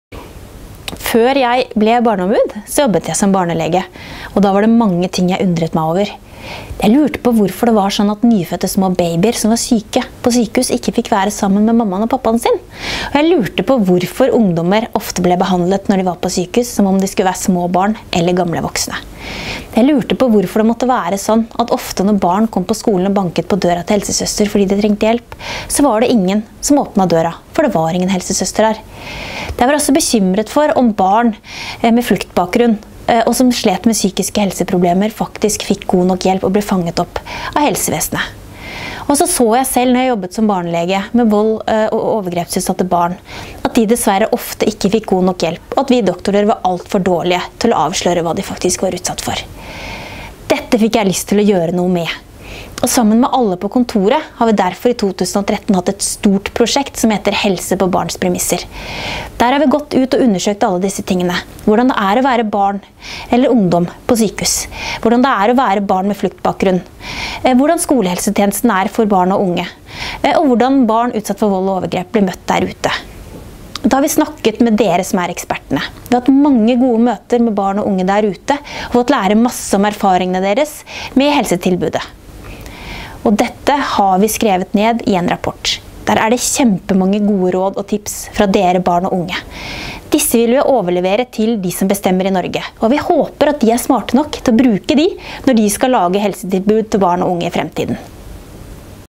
Introduksjon med Anne
Anne introduserer prosjektet helse på barns premisser. Video til lettlest versjon.